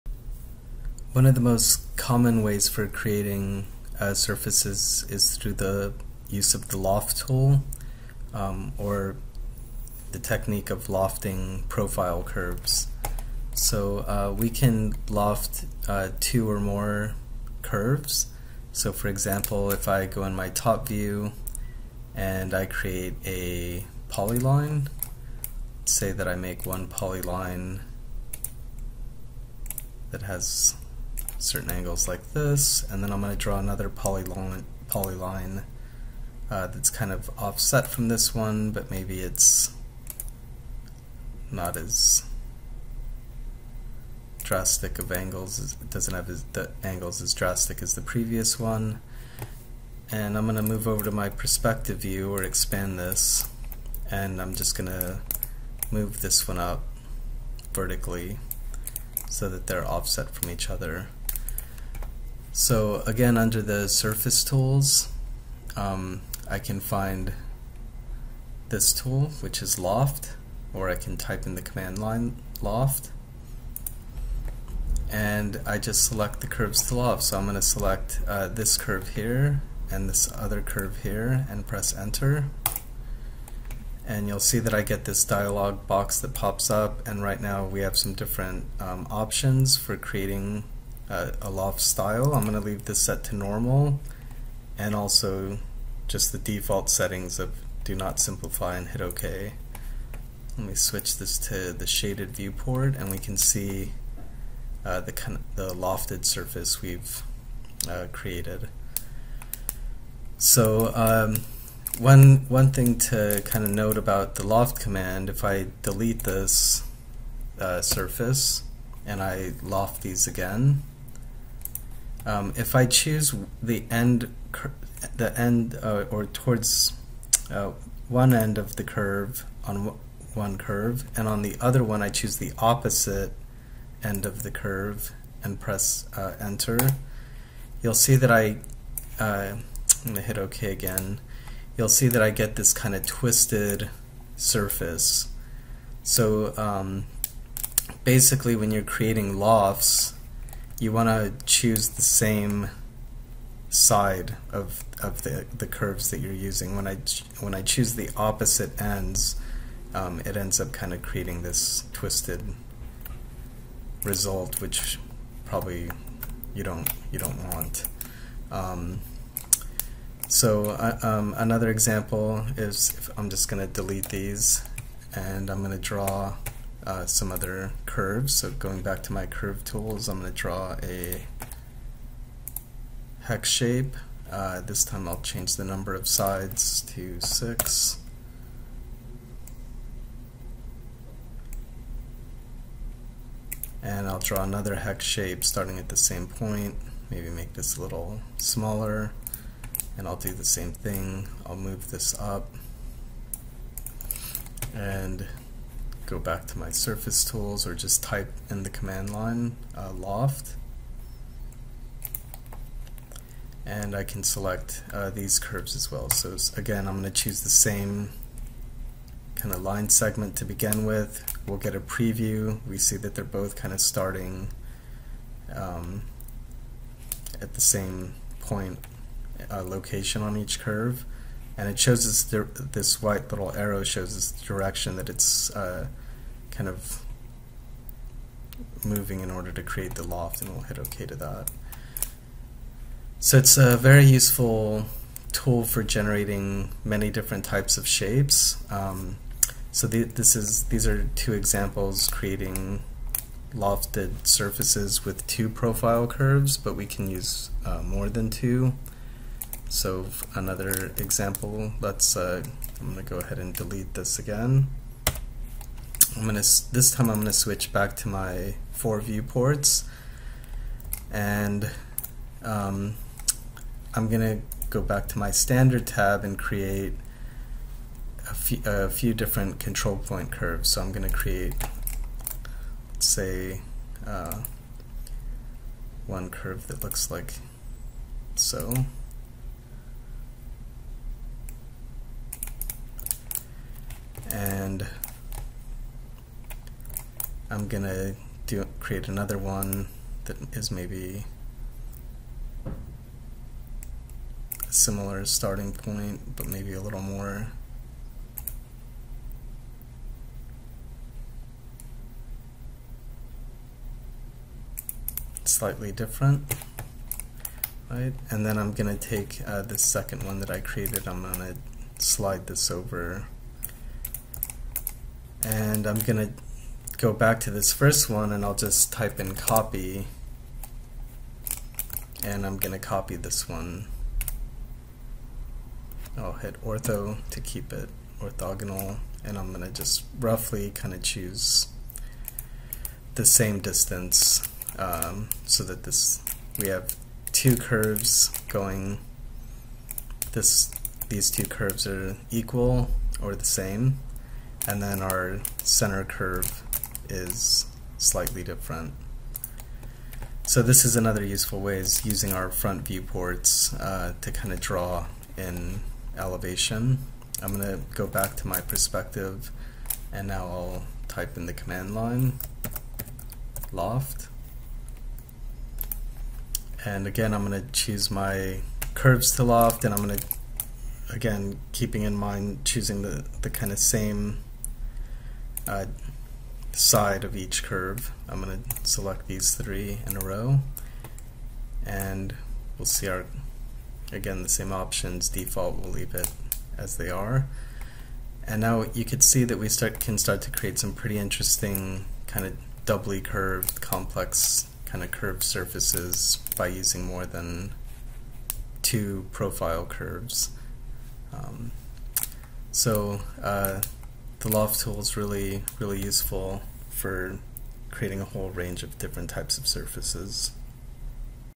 Rhino - 3D Modeling - Fundamentals - Drawing from the Model book_6.4.4_Surfaces: Loft
Creating surfaces using the loft tool to loft curves